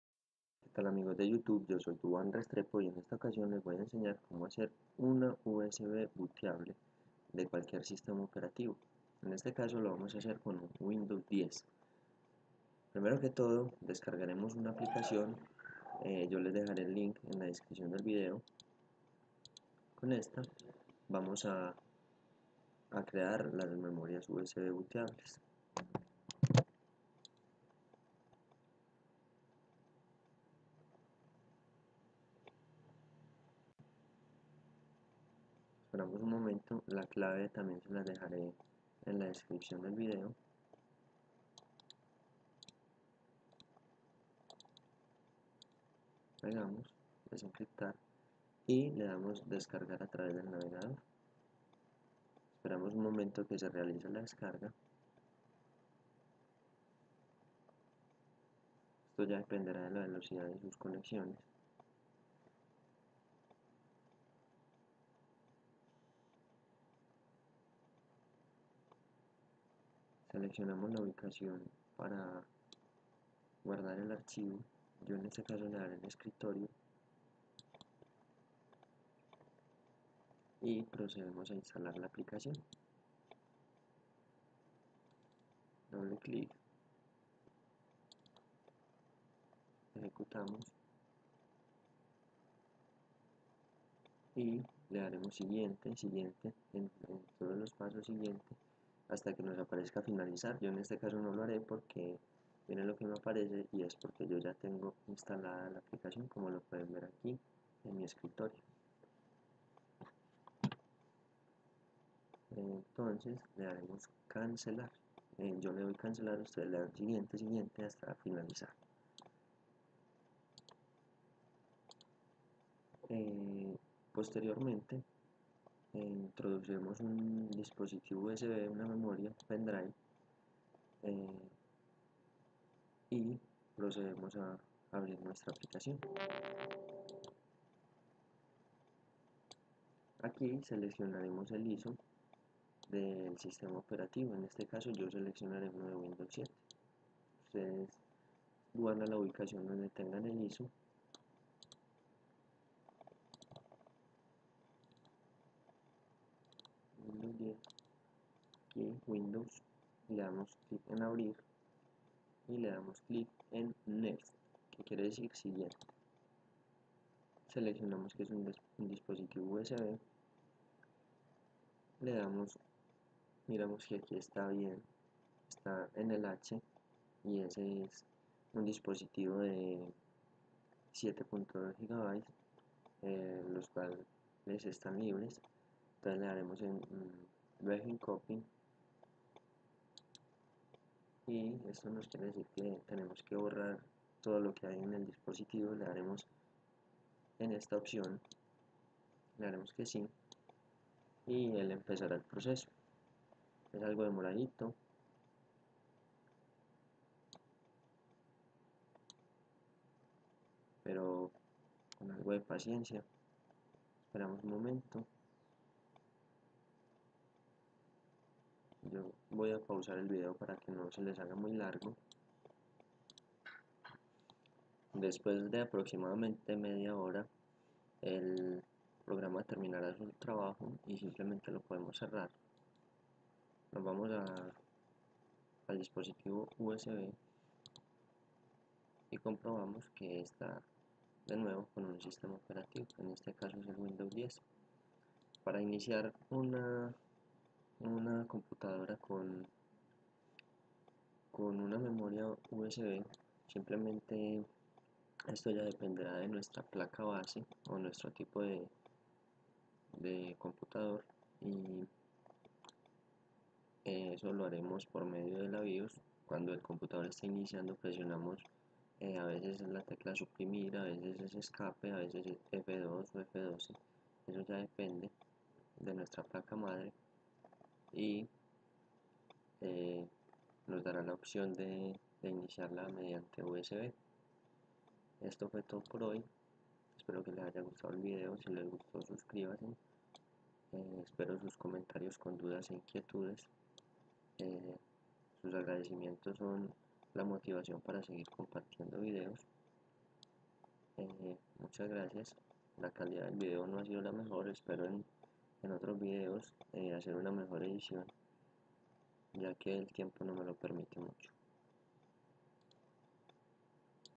Crear USB booteable windows 7, 8, y 10
Aprenda a crear un usb booteable con las isos de win  7, 8 o 10

clave: !ATBacXiAt1GQSQRv7kxo_tBr50EAOaVhmVOLtFQkyg0